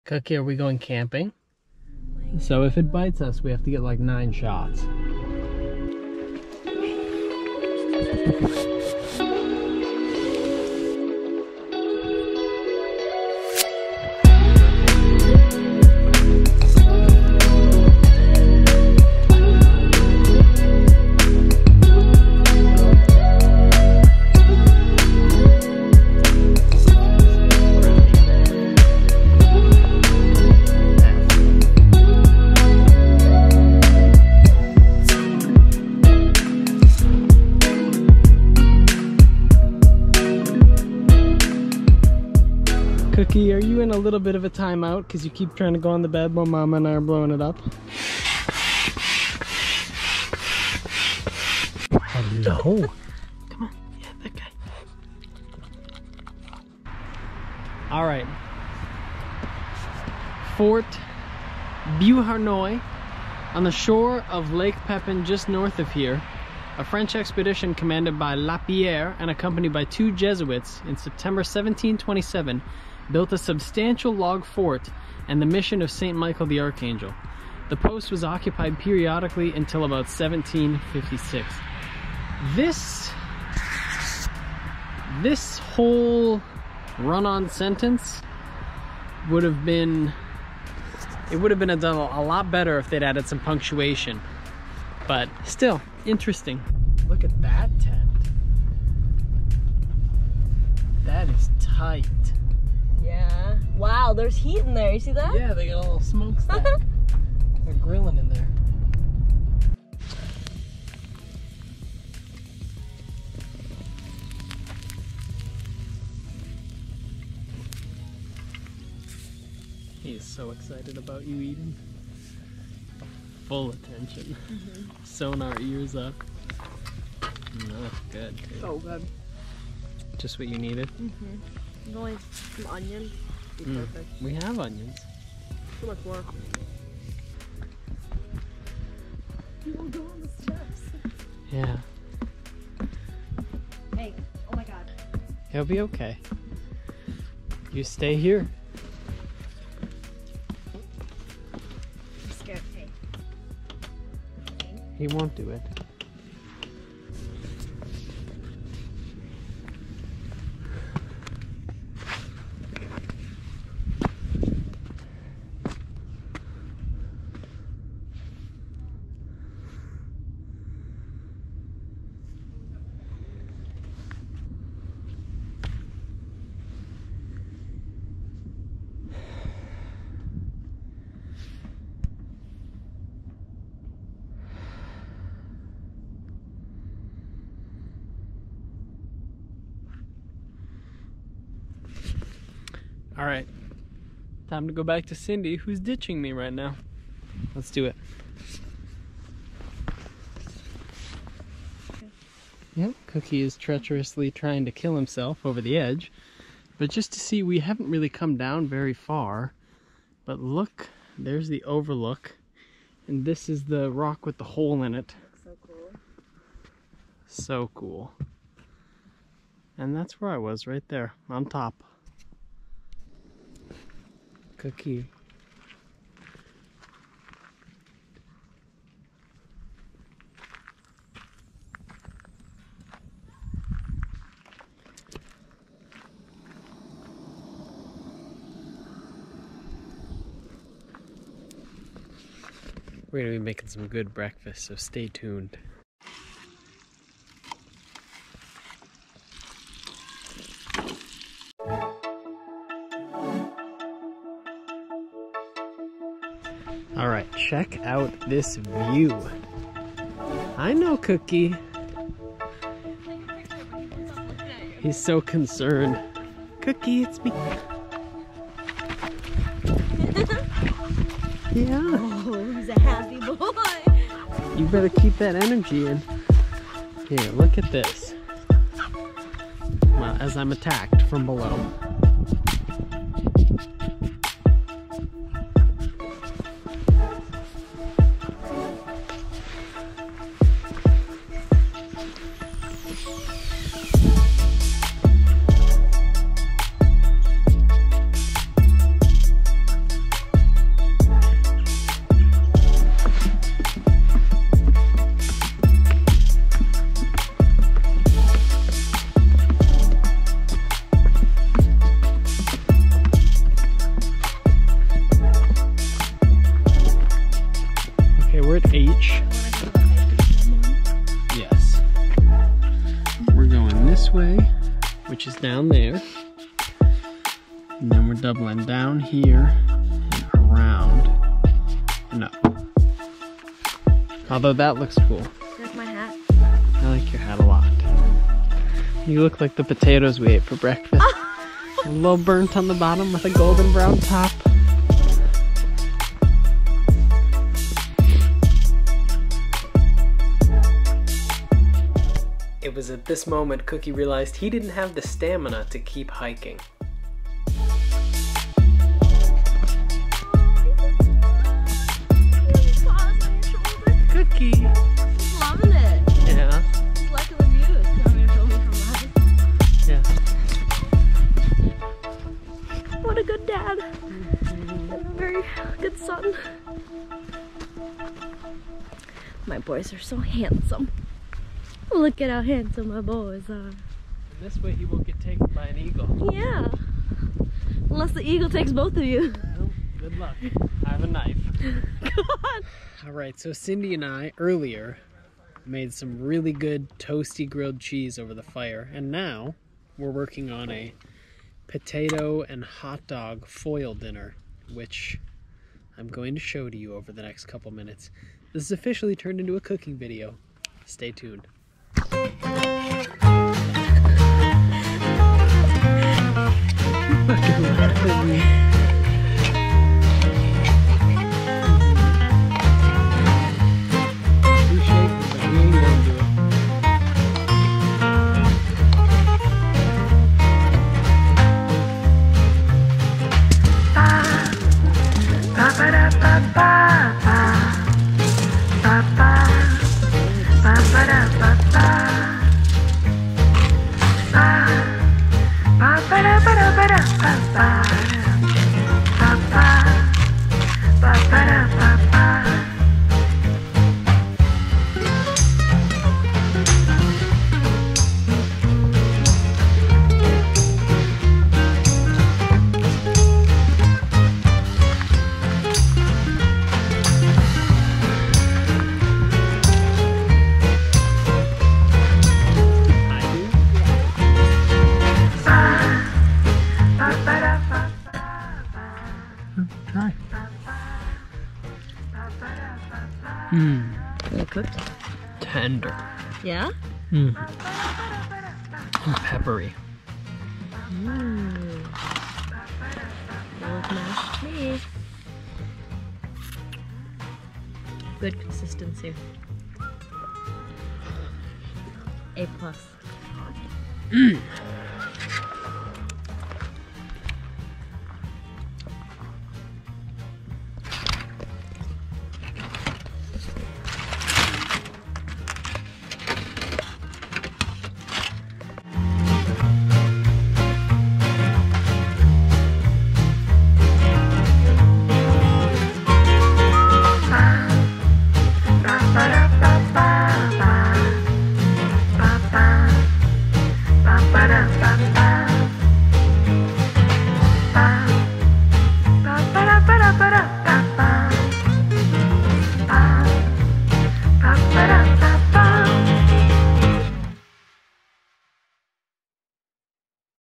French Bulldog Goes Camping - Cookie's Adventure
Cyndi and I took Cookie camping for his first time at Frontenac State Park just outside of Red Wing, MN.

Filmed on my Go Pro Hero 8 Black.